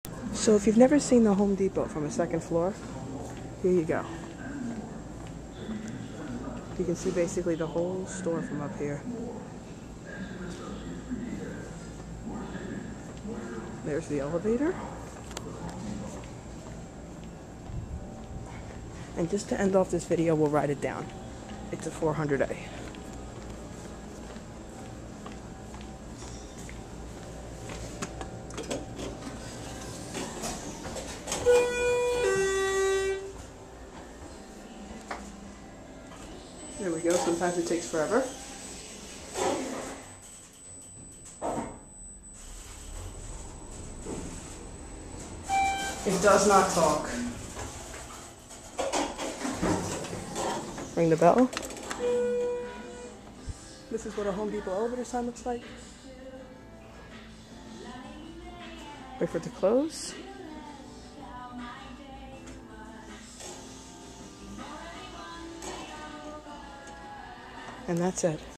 A View of The Home Depot from the 2nd Floor at Bellport Outlet Stores in Bellport, NY
This gives you a whole new perspective of the store.